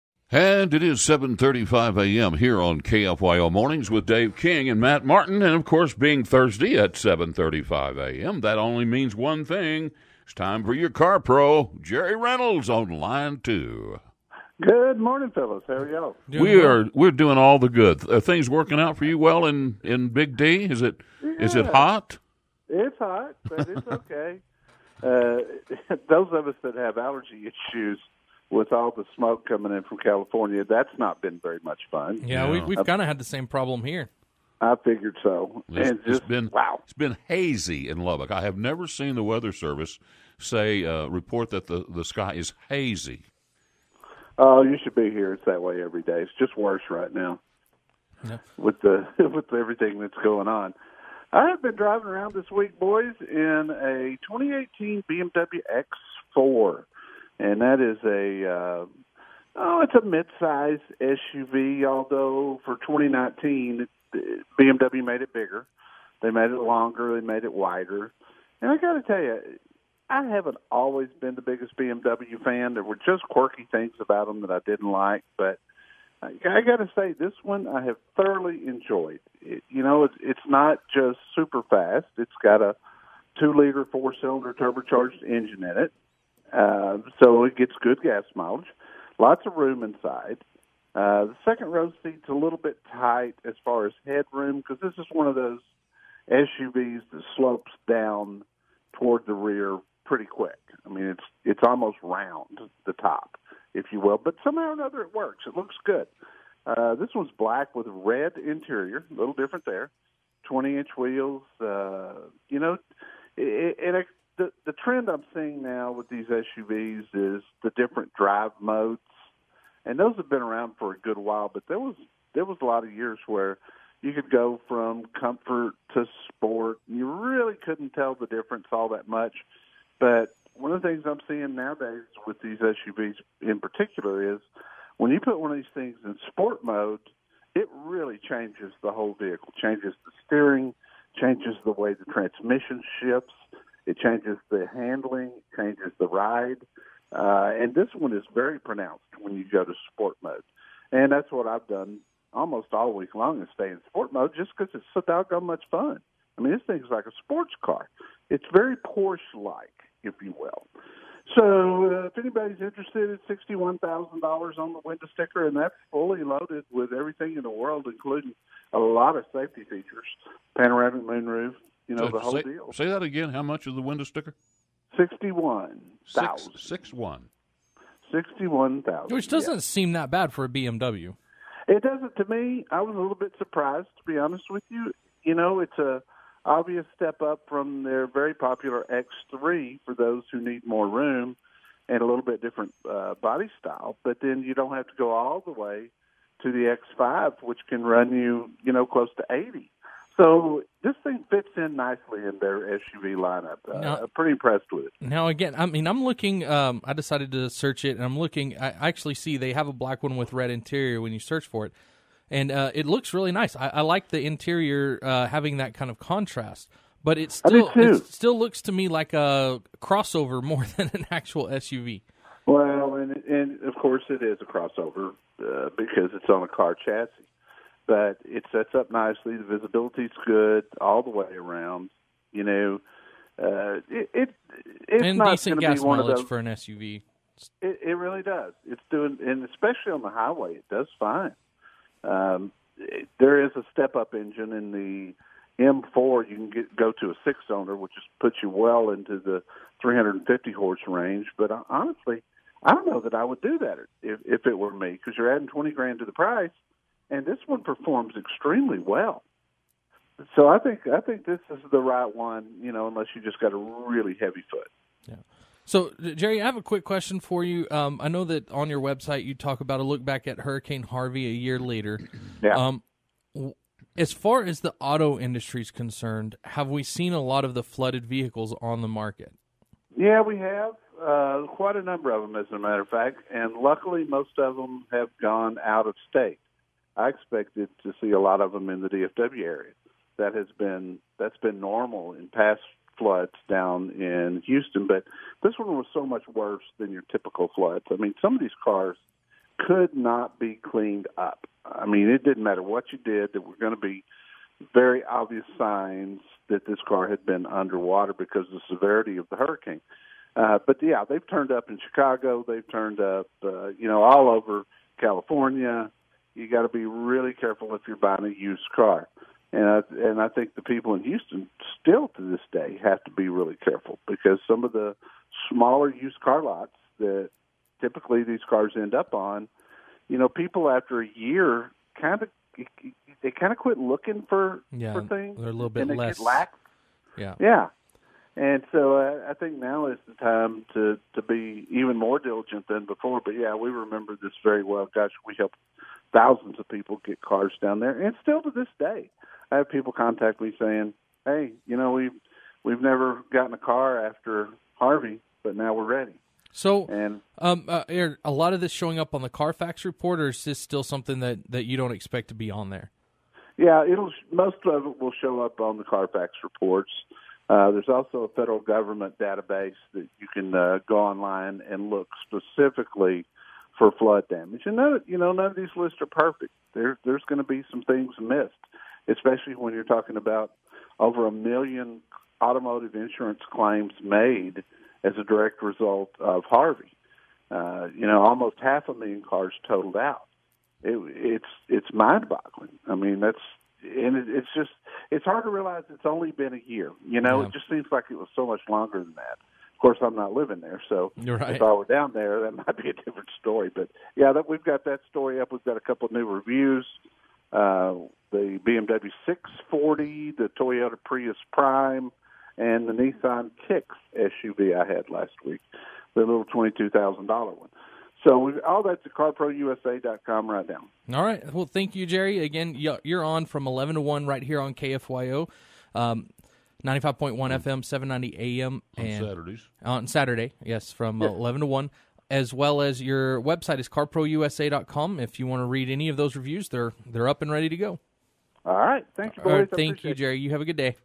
The Car Pro Jerry Reynolds Test Drives the 2019 BMW X4
"The Car Pro" Jerry Reynolds joins Dave King and Matt Martin to talk about his test drive of the week in the 2019 BMW X4 as well as other auto industry news. (August 23, 2018.)